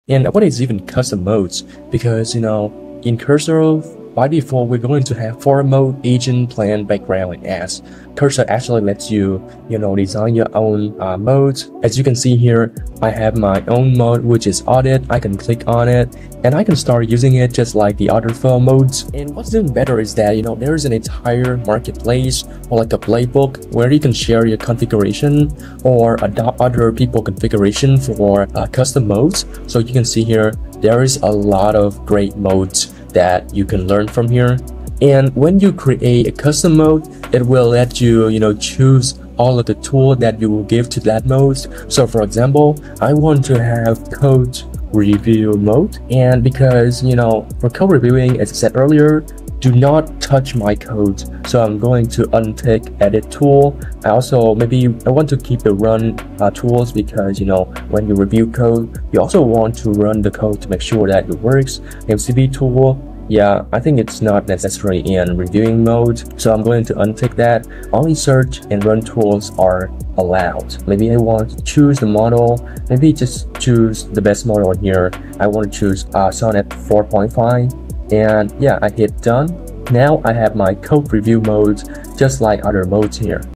Cursor Custom mode in UNDER 90 SECONDS
Create a customized mode in Cursor to 10x your AI coding workflow